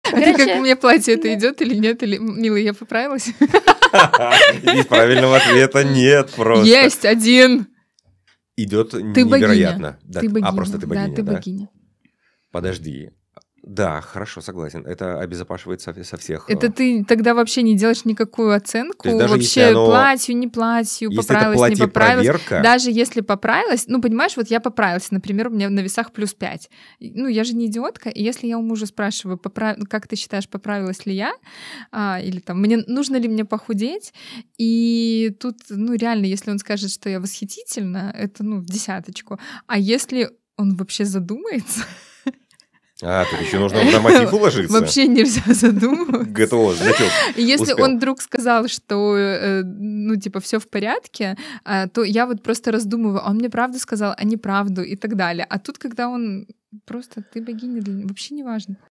Правильный ответ напишите в комментарии!😂
Евгений Перлин и Марина Фролова пытались найти правильный ответ 😂